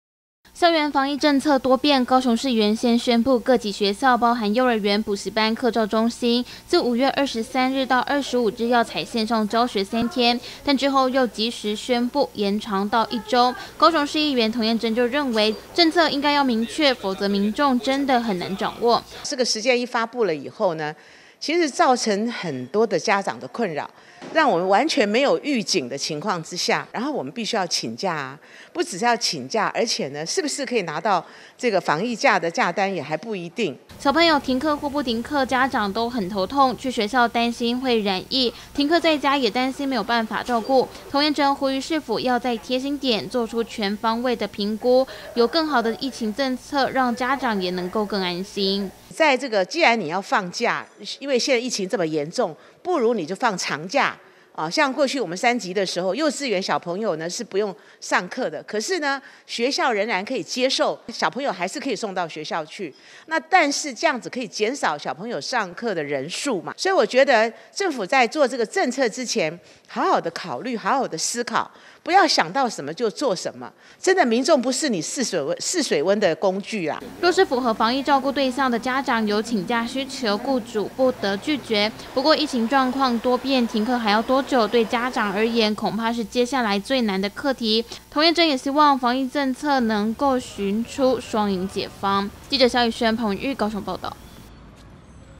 請假有難度|校園防疫政策多變 高市停課延長至七天
為了避免疫情擴散，高雄市宣布23日至29日高中以下停止實體課程，從原先的三天延長到了七天，對家長來說很頭痛，小朋友停課在家誰照顧？高雄市議員童燕珍就呼籲政府，再下達政策要明確才能夠讓家長有應變安排的時間。